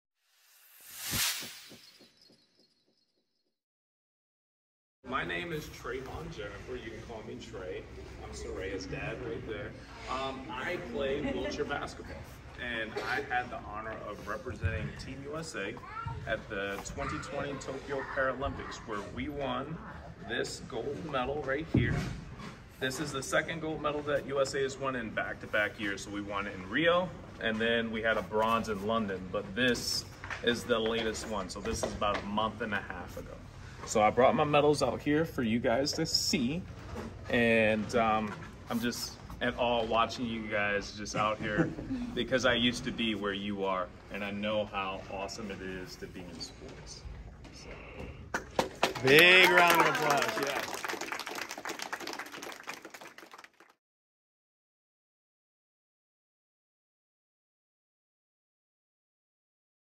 Paralympic Gold Medalist | Trevon Jenifer | Wheelchair Basketball
Trevon Jenifer — Paralympic wheelchair basketball two time gold medalist — spoke to the kids of Capital MMA.  Trevon had planned to go to college for wrestling, however, he ended up receiving an offer to attend Edinboro University to play basketball.  He represents Team USA and has participated in three Olympic Games (2012, 2016, and 2020).

His daughter is a member of Capital MMA and has been enjoying our children’s Gracie Jiu-Jitsu program.